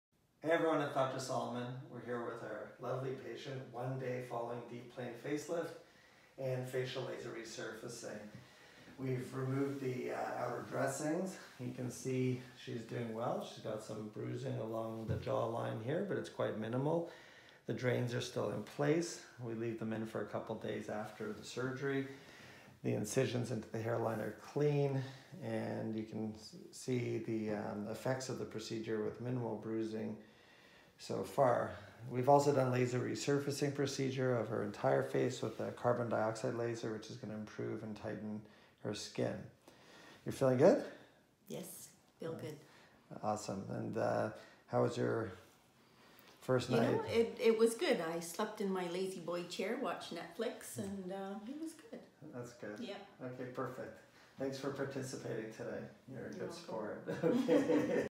Facelift Surgery Toronto | 1 Days Before & After Results | by Dr. Philip Solomon
This 57 year old female was in our clinic, the morning after her Deep Plane Facelift surgery. She also underwent CO2 Laser Resurfacing to further improve the texture and tone of her skin. Her dressing has been removed, her drains remain in place for up to 3 days.
▪️TREATMENT: DEEP PLANE FACELIFT / CO2 DEEP LASER RESURFACING
▪️COST: FACELIFT $16,500-$19,500 / CO2 LASER RESURFACING $3500
▪️PROCEDURE TIME: 4 HOURS
▪️DOWNTIME: 2 WEEKS
▪️Please consult a medical professional to assess your candidacy and to review risks. Results will vary amongst patients.

Dr. Philip Solomon MD, FRCSC
305 Davenport Road
Toronto, Canada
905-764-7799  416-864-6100